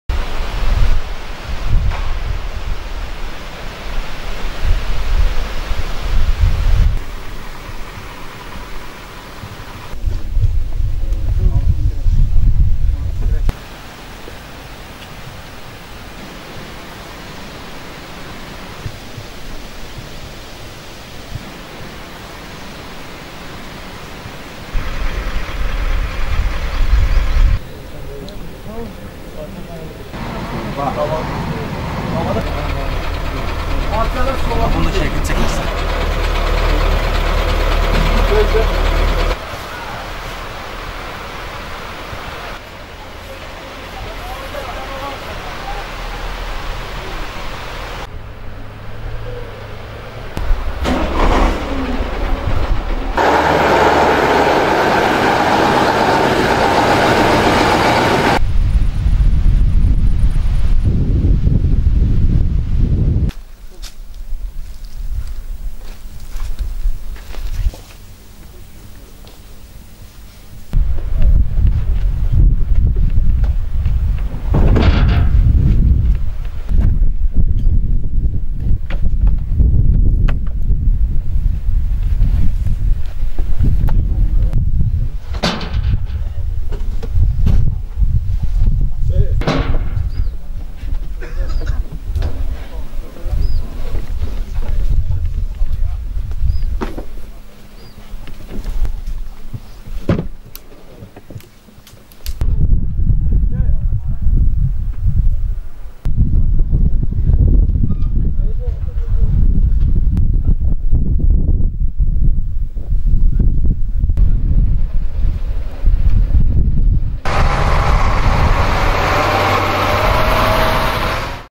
KƏLBƏCƏR
Kəlbəcər rayonu ərazisində növbəti dəfə döyüş texnikası müsadirə edilib#abuneol